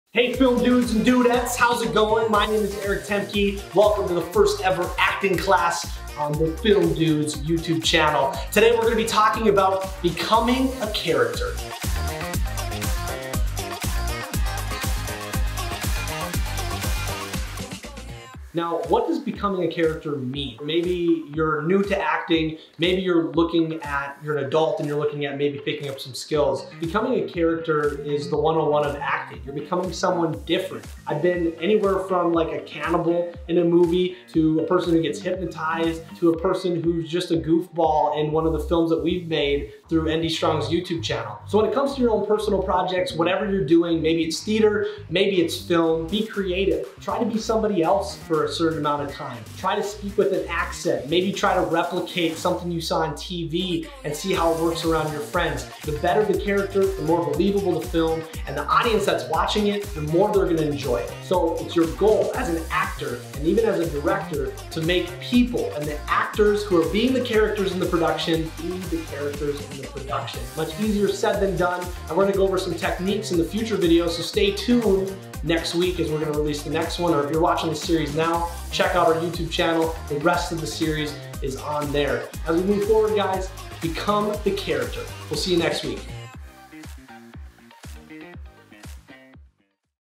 The Acting Class Series | Becoming a Character
The Acting Class Series is a mini-educational course hosted by Eric Thoemke, a film dude. The mini-course will cover the initial steps of acting and what it means to be an actor.

In this episode, Eric explains the importance of 'becoming the character' in your movie or theatrical production. 'Becoming a Character' seems simple, but many young actors forget that when they are acting they are leaving their own personality traits behind and becoming someone new.